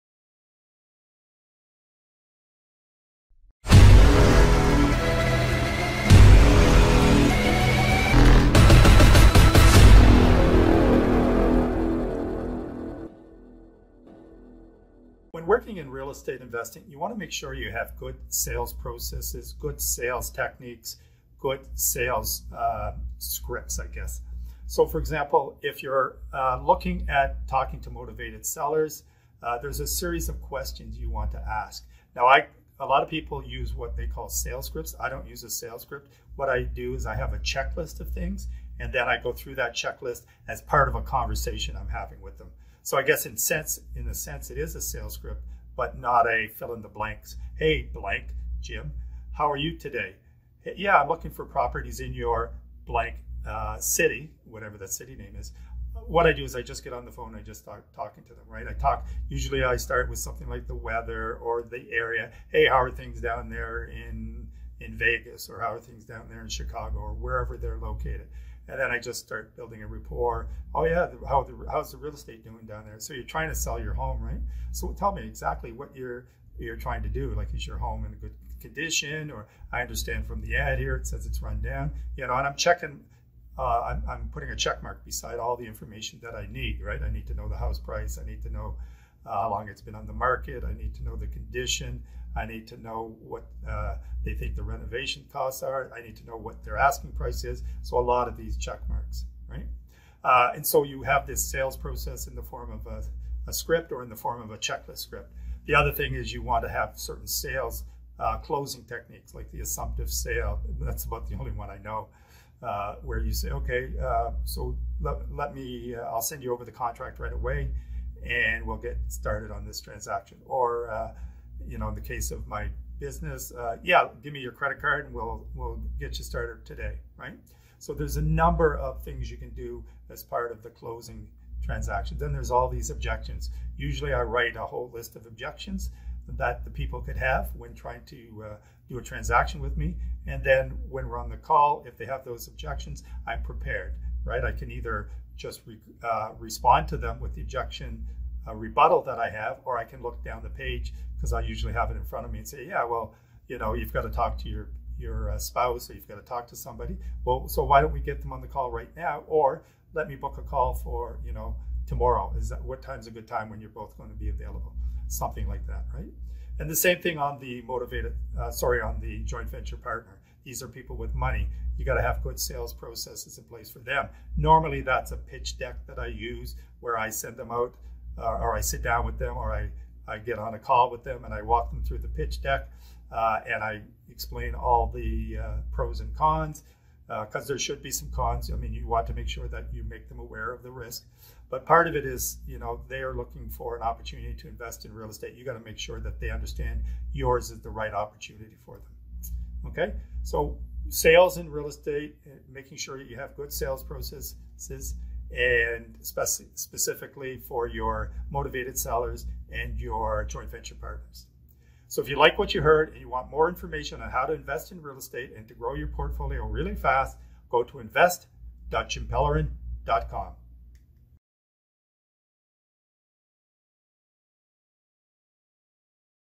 You Need Good Sales Tools and Techniques
If you would like to learn more about real estate investing and how you can scale your business and build a large portfolio fast ...  then click this link invest.jimpellerin.com

When working in real estate investing, you want to make sure that you have good sales processes, good sales techniques, and good sales scripts.

For example, if you are looking at talking to motivated sellers there are a series of questions you want to ask. Now, a lot of people use what they call sales scripts. I don't use a sales script. What I do is I have a checklist of things, and then I go through that checklist as part of a conversation I am having with them. So, I guess in a sense it is a sales script, but not fill in the blanks type sales script. “Hey, Jim [fill in the name], how are you today? I'm looking for properties in your blank.” Fill in the city with whatever that city name is.

What I do is I just get on the phone and I just start talking to them.

I usually start with something like the weather or the area. “Hey, how are things down there in Vegas?” Or “how are things down there in Chicago” or wherever they are located. And then I just start building a rapport. “How's the real estate doing down there. So, you are trying to sell your home right. So, tell me exactly what you are trying to do. Is your home in good condition or I understand from the ad, here it says it's run down”.

I am putting a checkmark beside all the information that I need.

I need to know the house price. I need to know how long it has been on the market. I need to know the condition. I need to know what they think the renovation costs are. I need to know what their asking price is.

I need a lot of these checkmarks, right? And so, you have this sales process in the form of a script or in the form of a checklist script.

The other thing you need is sales closing techniques.

Use the assumptive close, which is about the only closing I know. This is where you say, “okay I'll send you over the contract right away and we'll get started on this transaction” or in the case of my business yeah, “give me your credit card and we'll, we'll get you started today”

There are several things you can do as part of the closing transaction.

There will most likely be objections. Usually, I write a whole list of objections that the people could have when trying to do a transaction with me. And then when we are on the call, if they have those objections, I'm prepared.

I can either just respond to them with the objection rebuttal that I have, or I can look down the page because I usually have the list in front of me and say, “You've got to talk to your spouse. You've got to talk to somebody. Well, so why don't we get them on the call right now? What time a good time when you are both going to be available”, something like that. Right.

And the same thing with the joint venture partner. These are people with money. You've got to have good sales processes in place for them. Normally that is a pitch deck that I use that I send to them or I sit down with them or I get on a call with them and I walk them through the pitch deck.

What I do is I explain all the pros and cons and there should be some cons.

You want to make sure that you make them aware of the risk.

People are looking for an opportunity to invest in real estate. You have got to make sure that they understand yours is the right opportunity for them.

In real estate investing you want to make sure that you have good sales processes, especially for your motivated sellers and your joint venture partners.